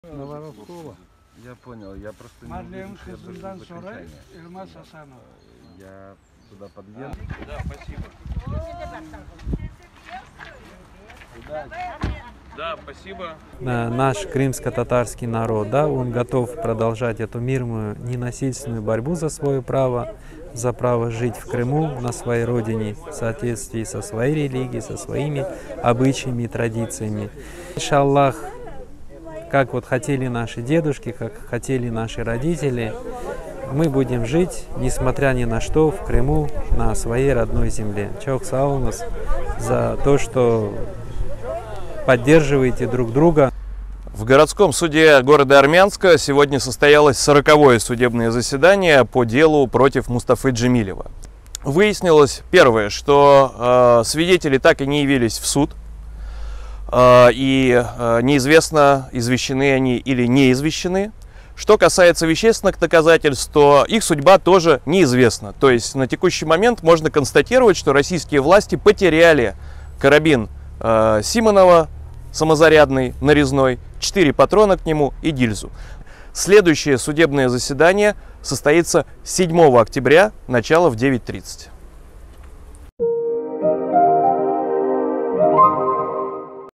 «СВИДЕТЕЛИ ТАК И НЕ ЯВИЛИСЬ В СУД, И НЕИЗВЕСТНО ИЗВЕЩЕНЫ ЛИ ОНИ», - адвокат Николай Полозов
15 сентября 2021 года в Армянском городском суде состоялся суд по «делу Мустафы Джемилева», в ходе которого рассматривалось дело по существу.

Подробнее в нашем видеосюжете.